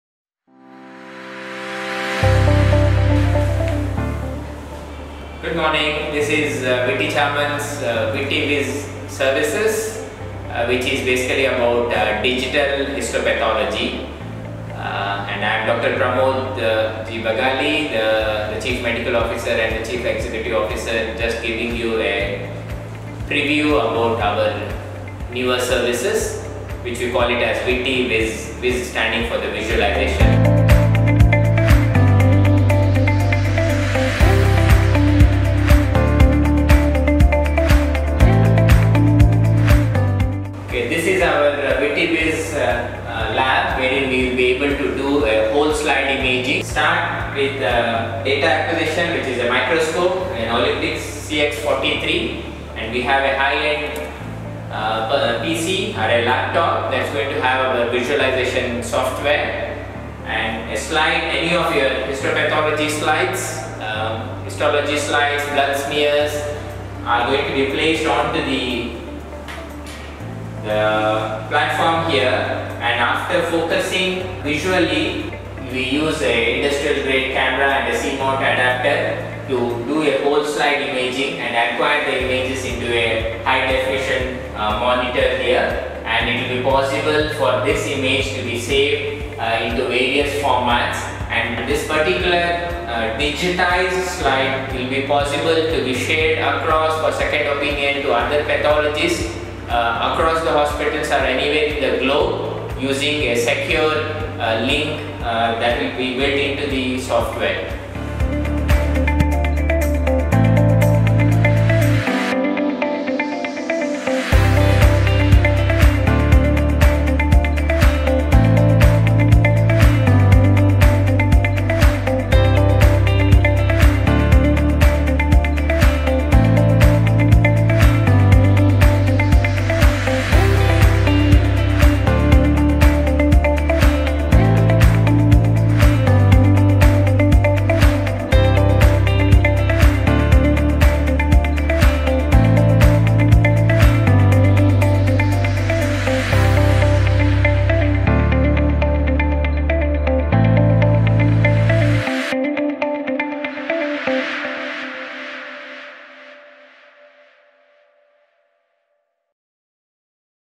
Whole Slide Imaging (WSI) Digital Slide Scanning with Witty Viz by Witty Charman
Whole slide imaging (WSI), refers to HD scanning of the conventional glass slides in order to produce digital slides, is the most recent imaging technology adopted by the Pathology departments worldwide. It serves its significance for empowering diagnostics, educational and research purposes.

The WittyVizTM system able to quickly captures high resolution digital image of the entire slide and adding efficiency by annotation, storage, retrieval, analysis, cross consultation and other sharing via a secure portal where you would need not worry of the storing of your patient’s confidential data.